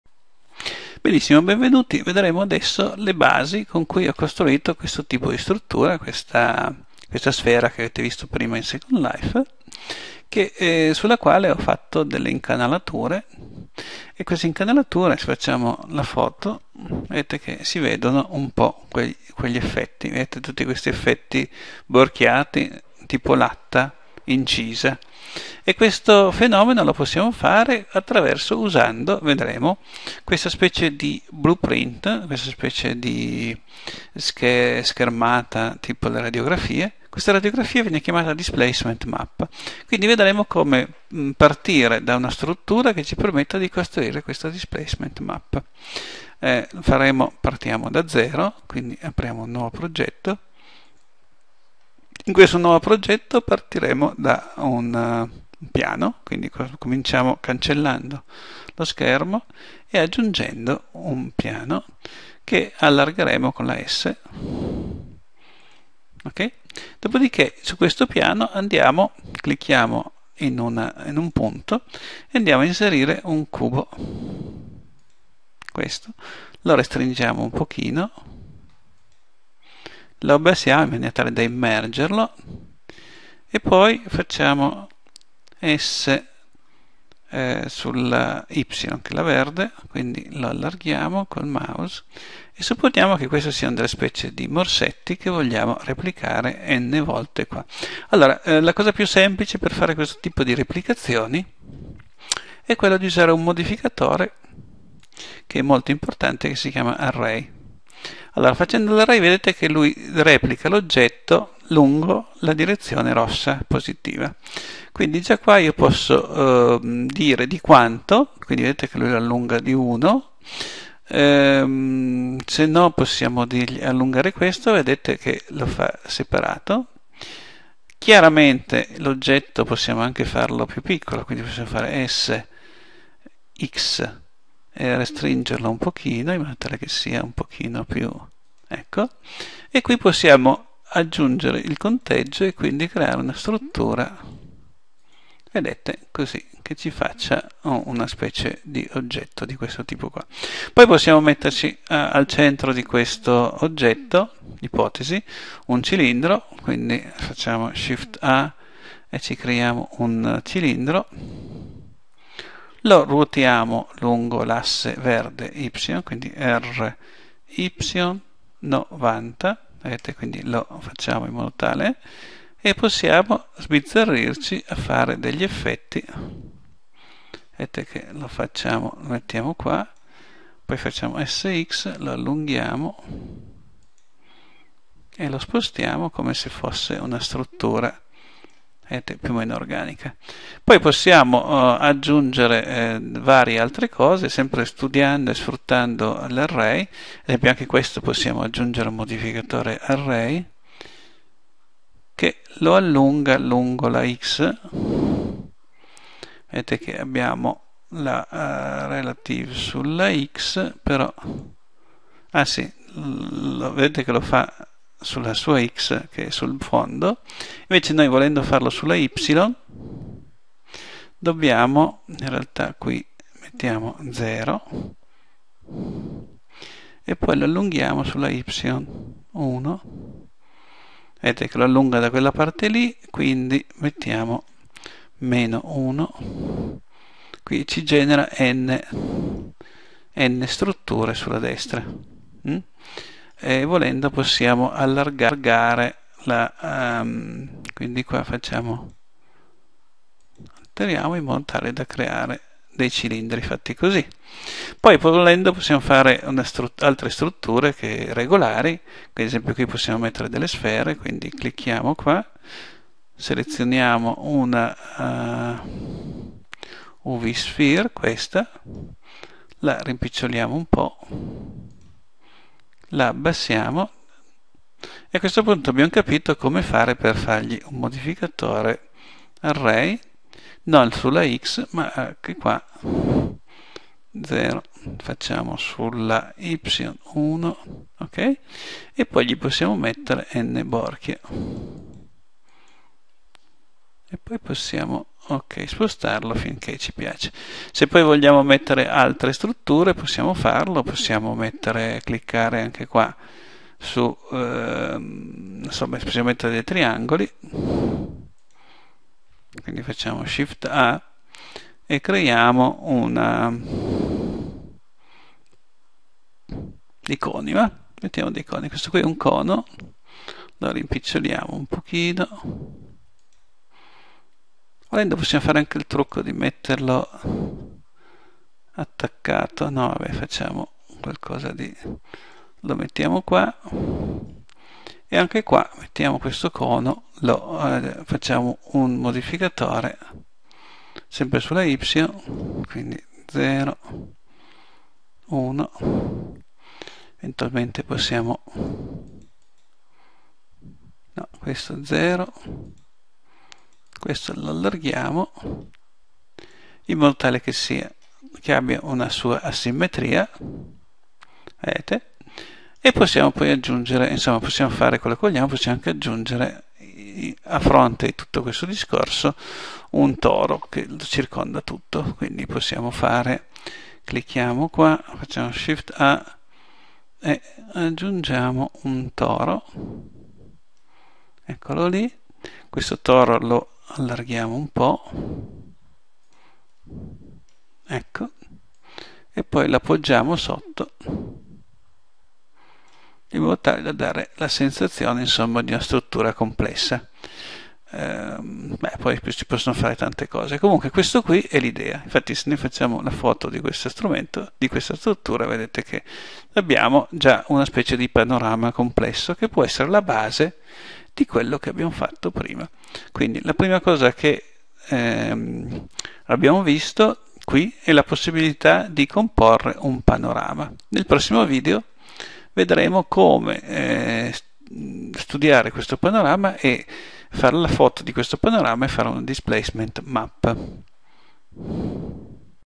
displacement map pt 1
Dopo l'introduzione alle normal maps, prima lezione che insegna come si possono costruire delle displacement map partendo da "panorami" geometrici.